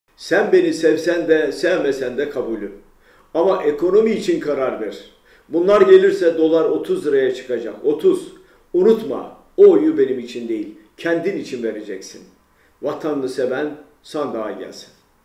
Ekonomik yıkımı yaşayan güzel ülkemizi son çıkışta kurtaracak mıyız...
Bu bir referandumdur. Ekonomik yıkımı yaşayan güzel ülkemizi son çıkışta kurtaracak mıyız, yoksa uçurumdan aşağı yuvarlanacak mıyız?
Vatanını seven sandığa gelsin!